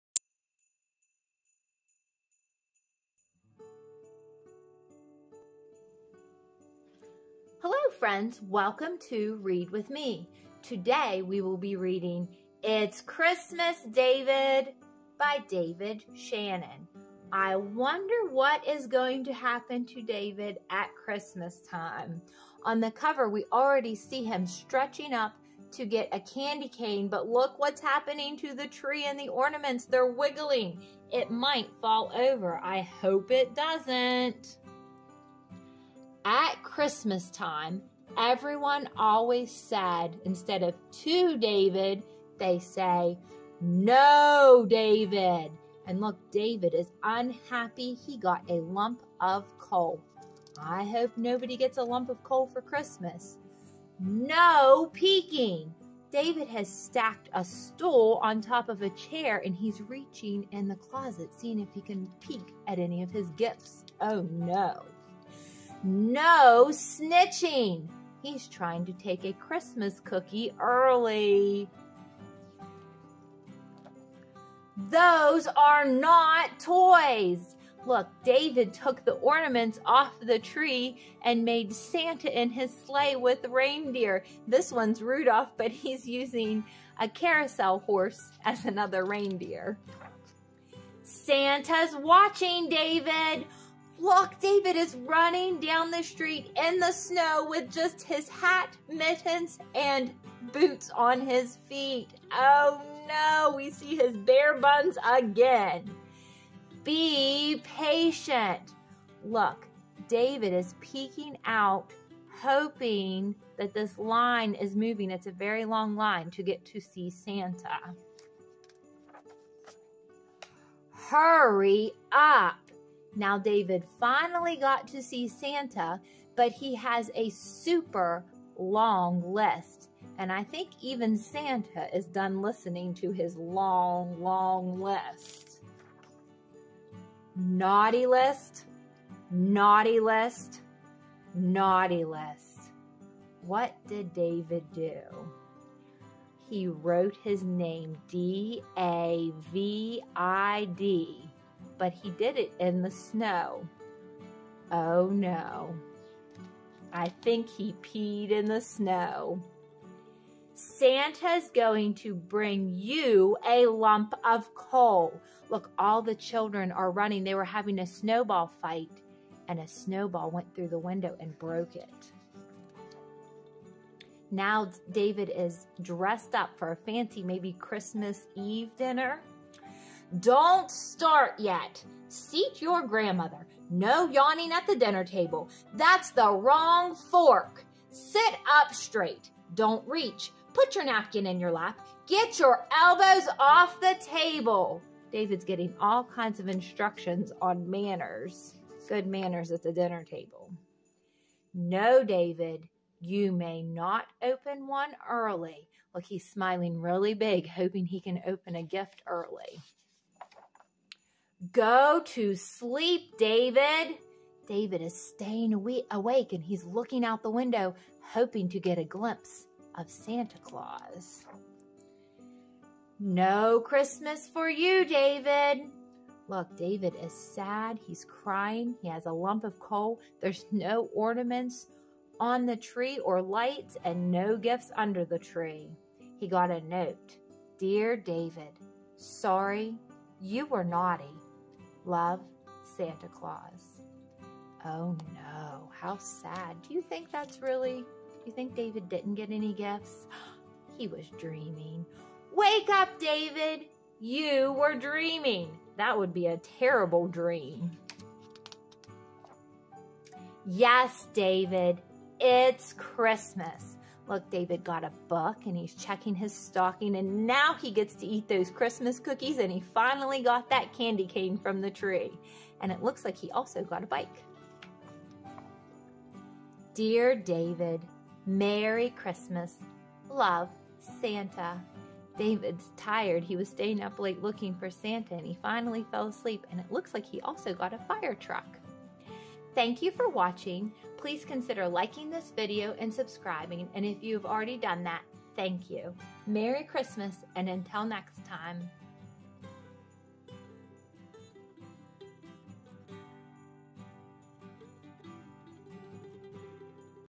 It's Christmas, David! by David Shannon Read Aloud Book
A read aloud of the children's book, "It's Christmas, David!" by David Shannon.  David gets into a lot of mischief waiting for Christmas! Will Santa visit his house on Christmas Eve? Read With Me to find out!